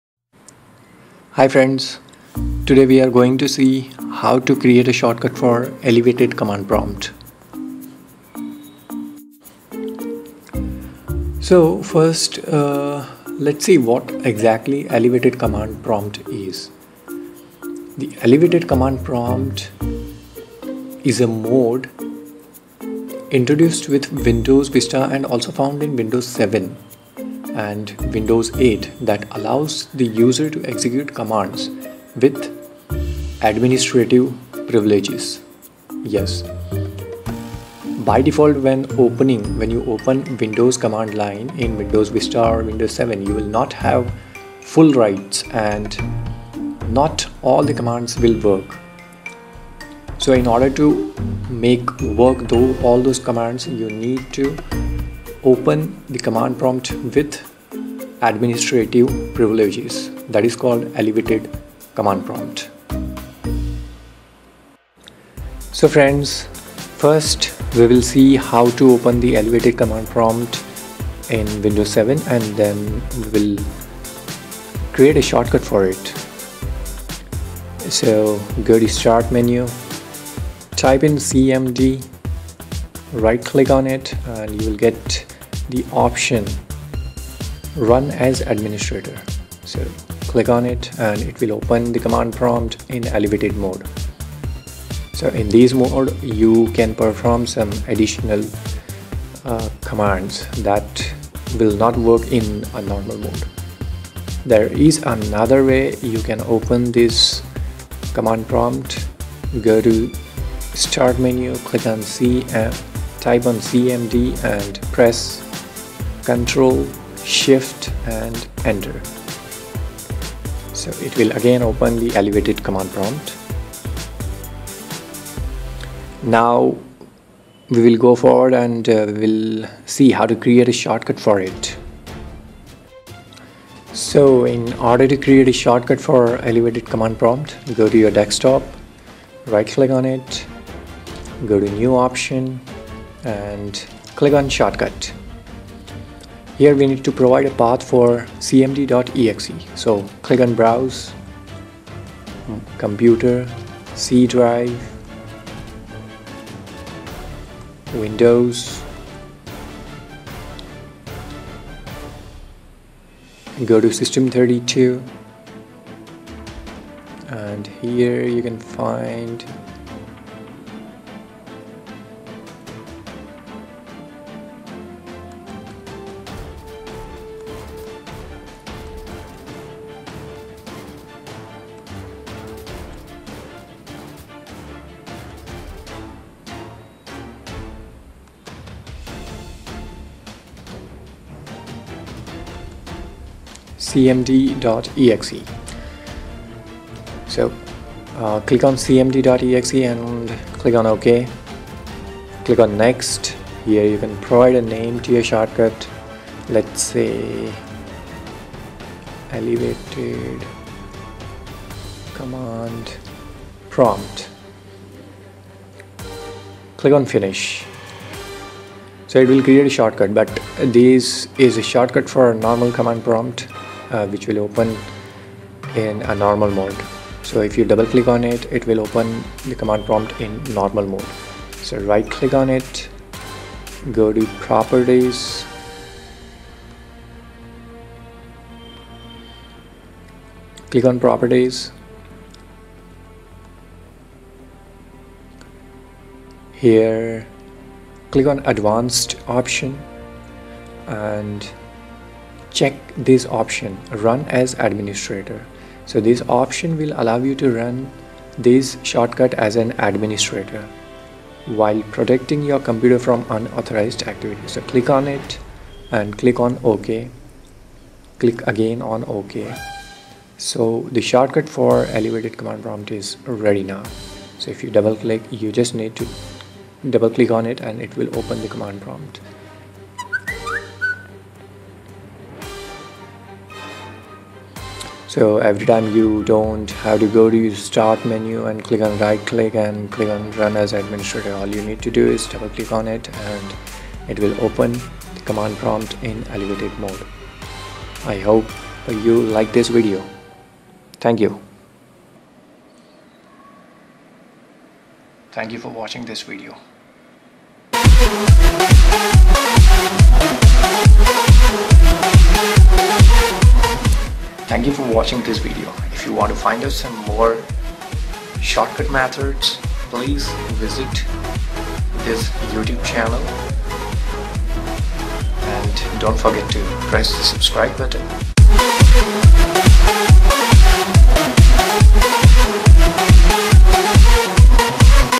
Create a Shortcut for Elevated Command Prompt : Run As Administrator
Here is a trick to create a Shortcut for Elevated Command Prompt.

Elevated Command Prompt : Its a mode where you can perform some additional tasks with Administrator Privileges. Some of the commands never works in normal mode, so we have to use this mode in order to run those commands.

Generally we have to go to the CMD and then Right Click on it and then Click on "Run As Administrator". But instead, you can create a shortcut for it and place it on your Desktop so that you can just double click on the short cut and it opens the elevated command prompt.

The following feature was introduced in Windows Vista.